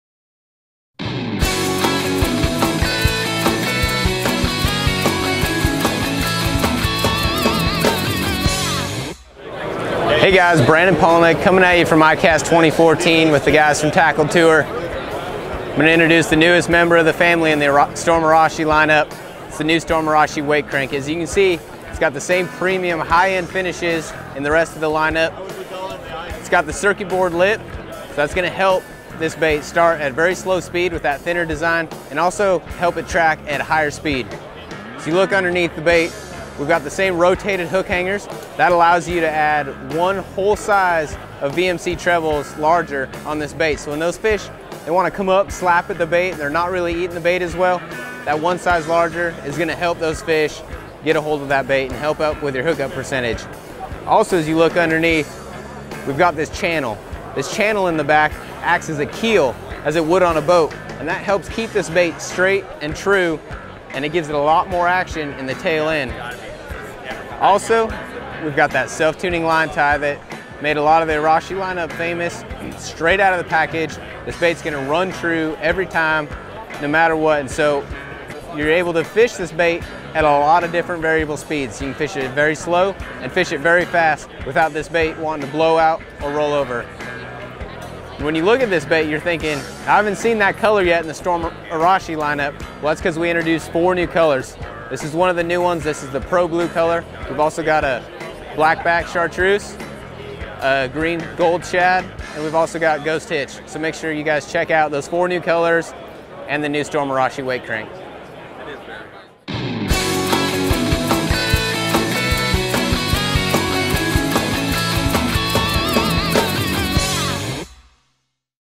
Storm Arashi Wake Crank
Pro Angler Brandon Palaniuk introduces the new Storm Arashi Wake Crank. This smallish bait packs a good casting punch weighing in at just over half an ounce. It features a circuit board lip, holographic 3D eyes, VMC Black Nickel Hooks, rotated hook hangers, and of course, that patent pending self tuning line tie. It will be available in thirteen (13) different colors.